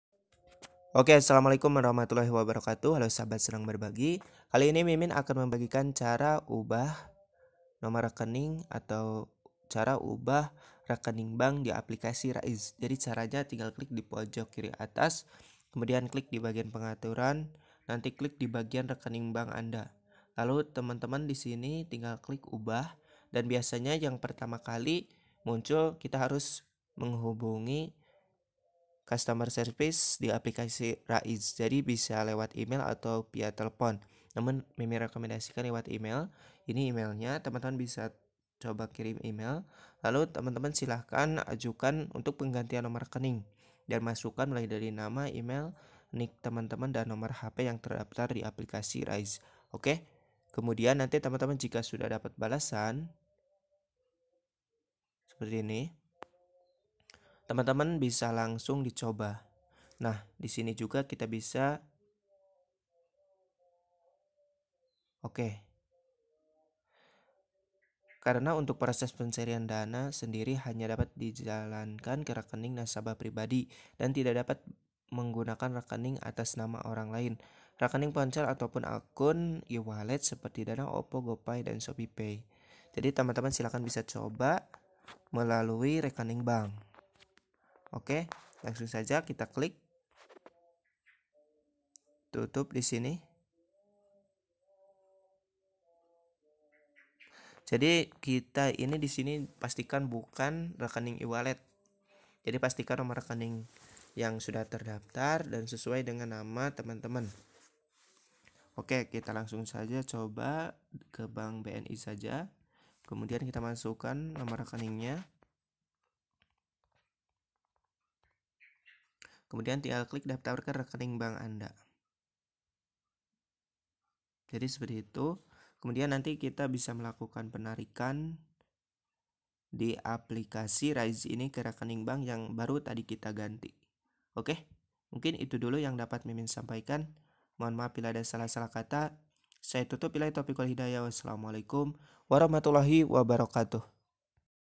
CARA UBAH REKENING BANK DI APLIKASI RAIZ
CARA UBAH REKENING BANK RAIZ
1. Login ke Aplikasi Raiz.
2. Klik menu Pengaturan
3. Klik menu Akun Bank
4. Klik Ubah
5. Pilih rekening Bank baru yang ingin didaftarkan
6. Masukkan detail rekening
7. Lakukan konfirmasi perubahan rekening